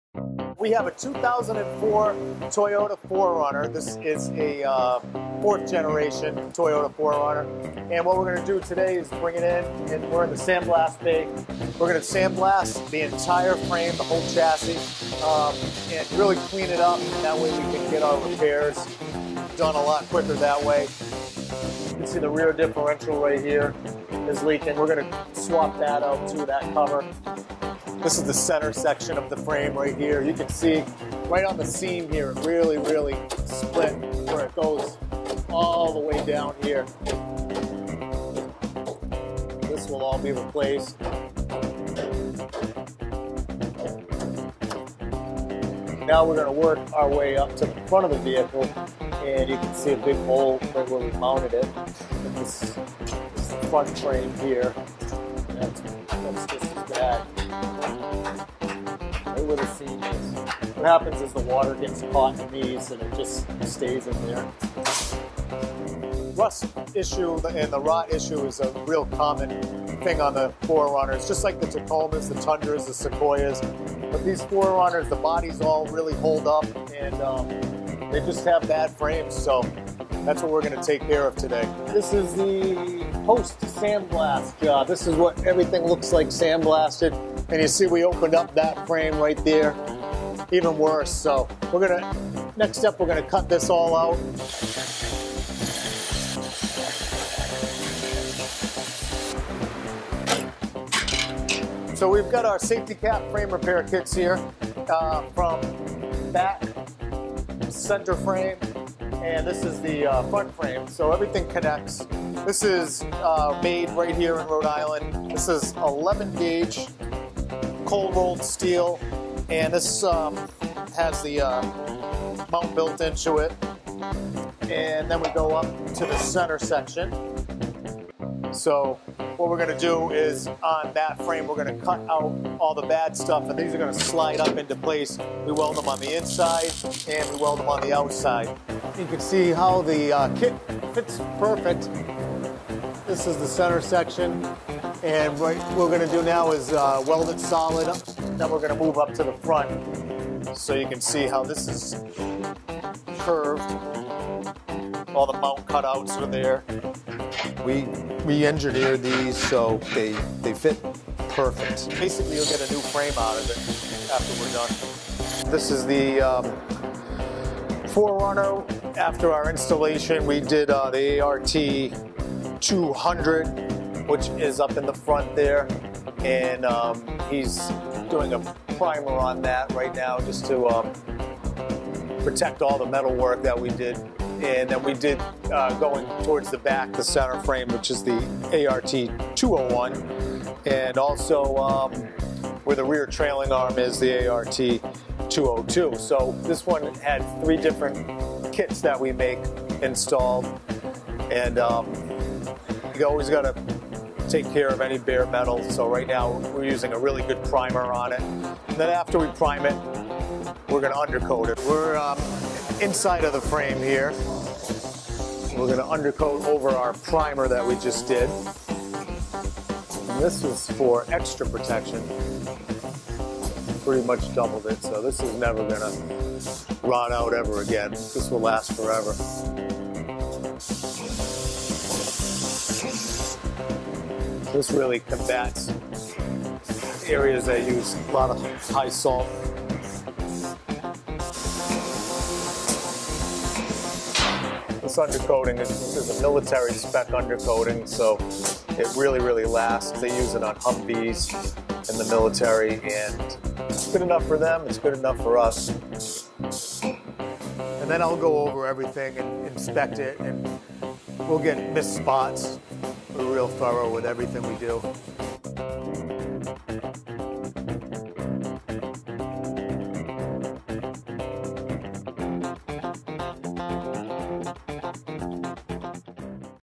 SafeTCap® Install on 4-Runner
Auto Rust Technicians installing SafeTCap® Frame repair kits on the 4th gen(03-09) Toyota 4-Runner.